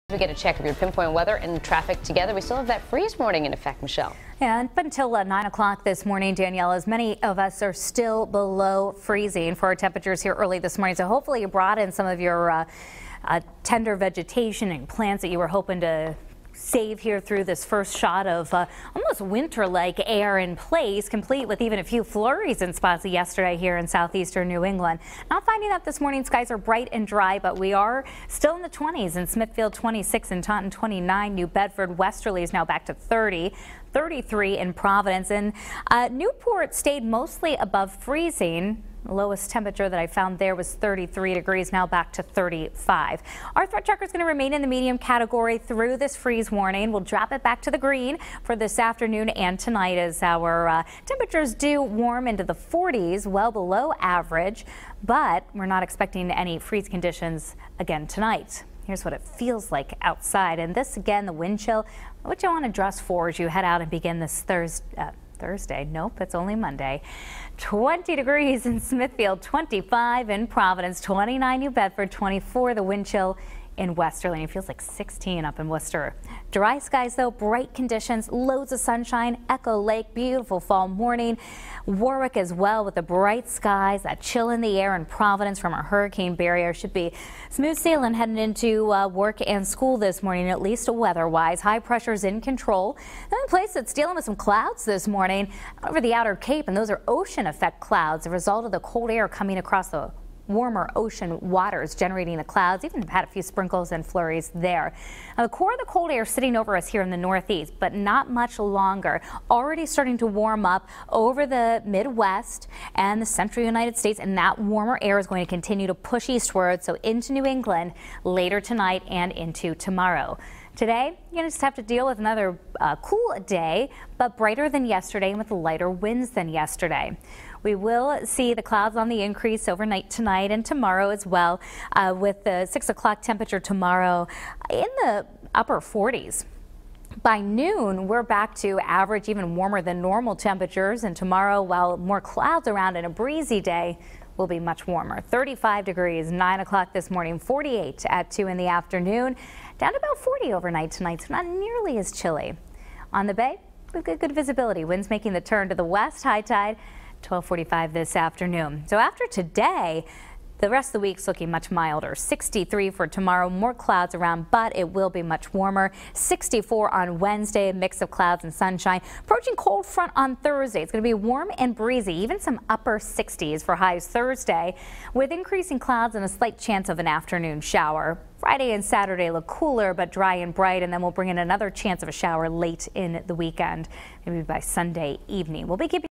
Live Pinpoint Doppler 12 Futurecast
Eyewitness News Meteorologist Michelle Muscatello has the morning weather for Monday, October 19, 2015